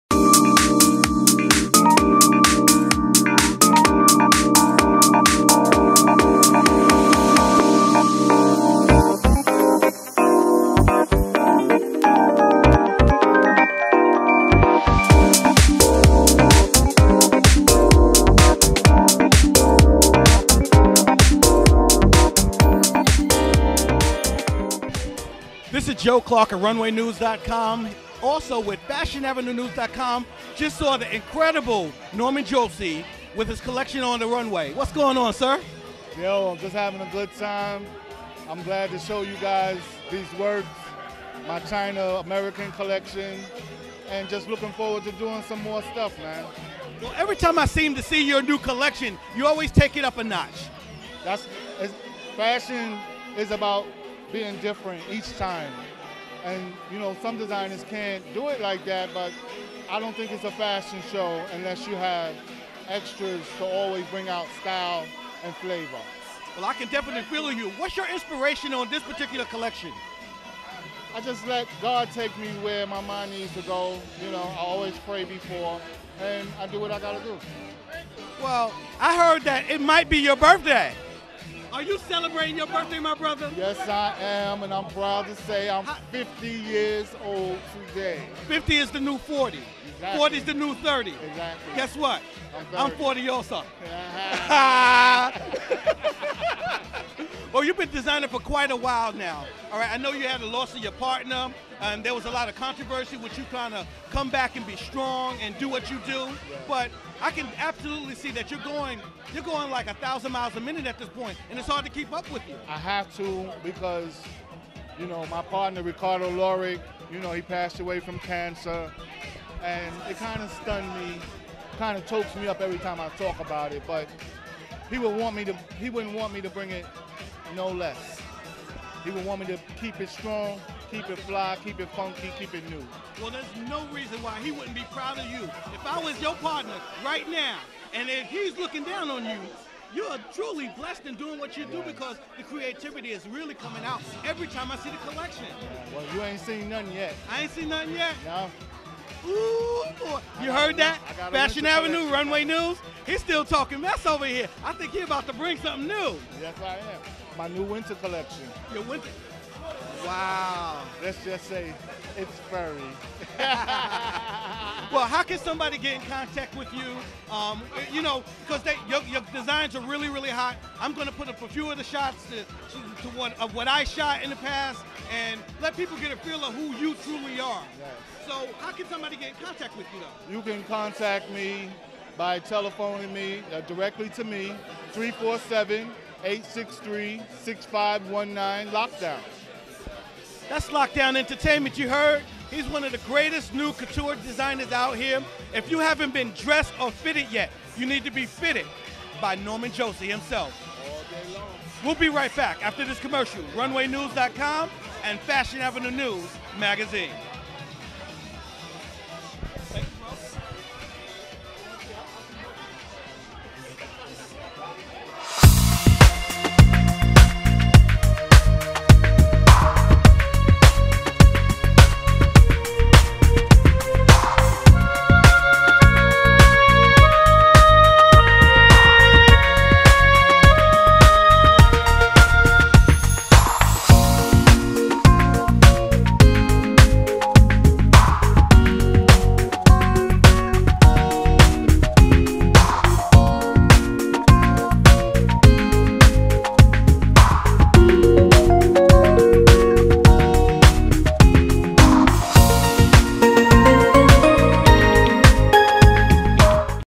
Norman Josey Interview at the Time Hotel
Fashion Avenue News Magazine Release & Networking Party in NYC at the Time Hotel.

Interviews by: RunwayNews.com